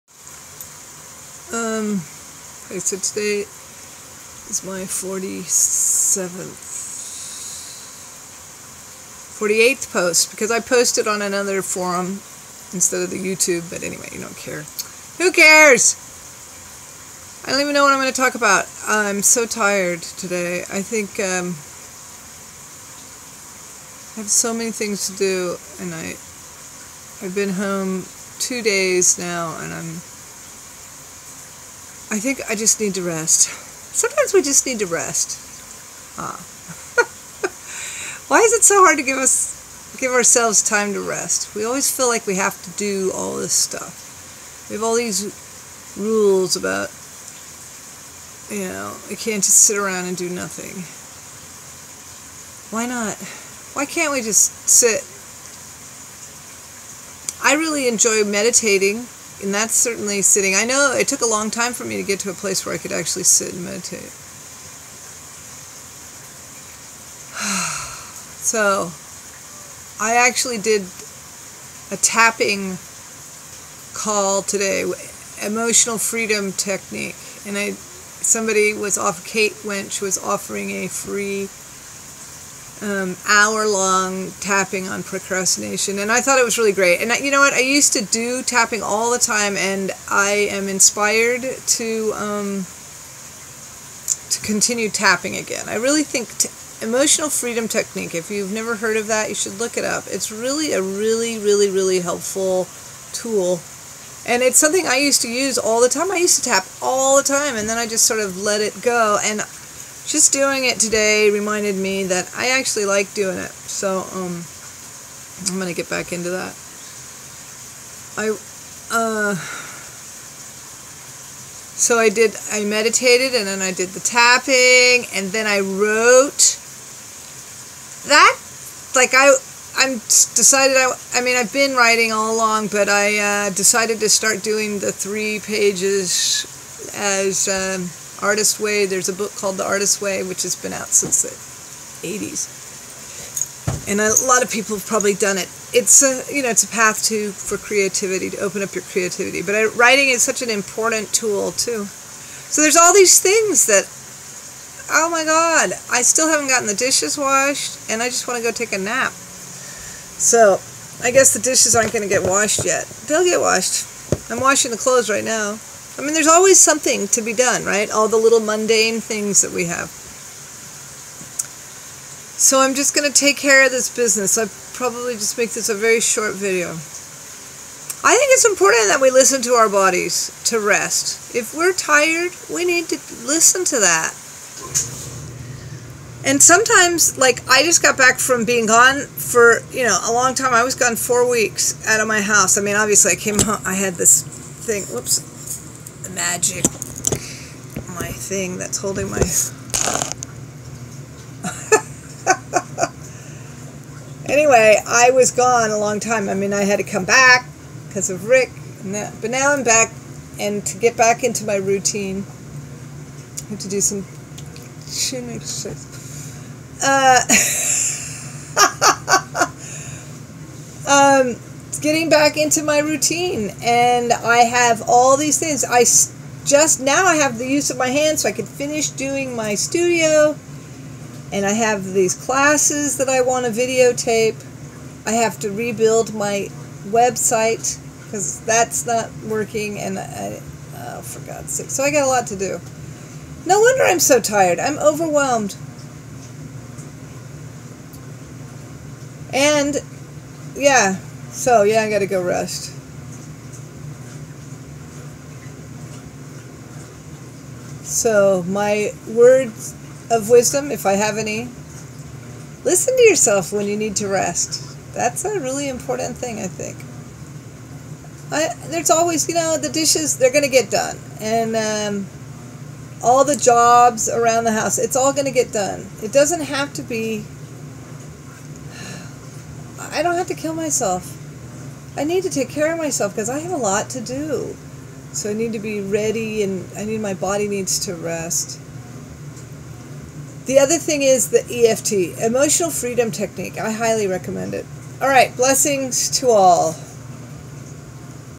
Confessions of a Sacred Prostitute - Day 48
Emotional fReedom Technique, and the importance of rest.  July 22, 2017